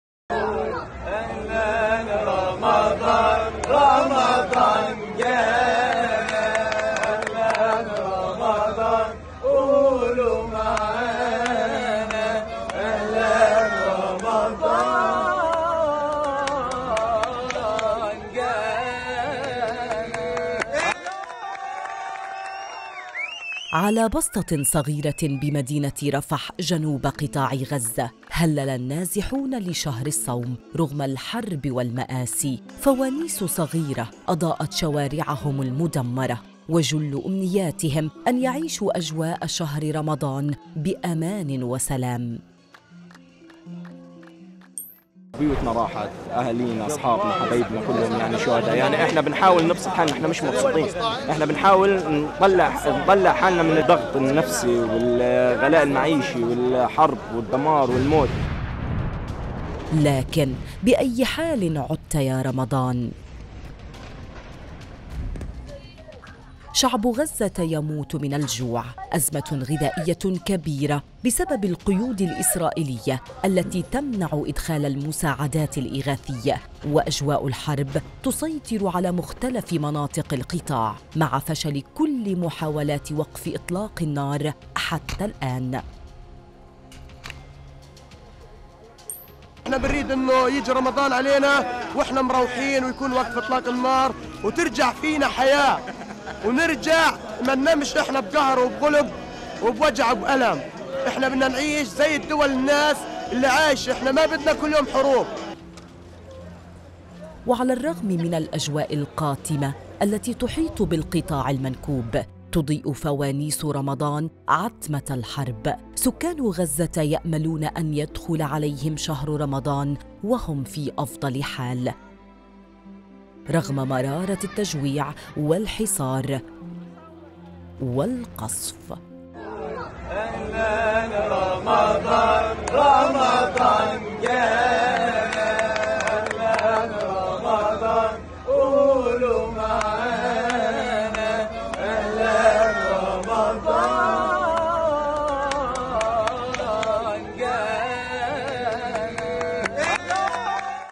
فوانيس رمضان حاضرة على أرصفة غزة المدمرة
رغم الجوع والحرب والدمار.. فوانيس رمضان حاضرة على أرصفة غزة المدمرة

لمعرفة أحدث المستجدات.. عبر البث المباشر من هنا ⤵
وبإمكانكم متابعة العربية على منصات التواصل